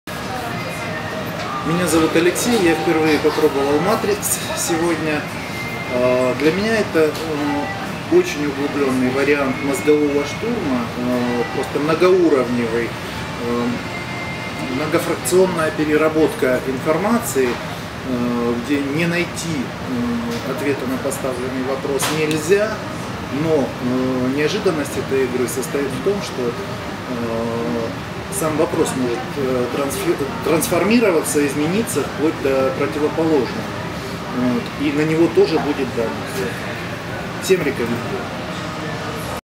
MatriX в Ростове Отзыв6
MatriX в Ростове - на - Дону
1. Что такое «MatriX», если коротко?
MatriX – настольная игра, на 8 человек.
Формат – креативная сессия, наполняющая творчеством процесс совместной деятельности – от обсуждения стартапов до реализации проектов или проведения совещаний.
Сфера применения – маркетинг, реклама, PR, социальные и бизнес – проекты. Цель – поиск новых идей и их сочетаний, с реализацией в конкретных действиях.
2. В чем уникальность?
«MatriX» позволяет сразу погрузиться в конкретный запрос, актуальный для участников и начать работать непосредственно с ним. Сколько вы знаете процессов, которые можно запускать «сразу в дело»? Вот-вот, я тоже немного))
3. Каковы выгоды для участников открытого формата?
Кубик и игровой подход создают состояние «творческого ребенка», в котором качество идей на порядок выше, чем при стандартных подходах, у участников прекрасная возможность не только найти собственные решения в теме игры, но и прокачать свое «умение мыслить за пределами привычных шаблонов». Подробности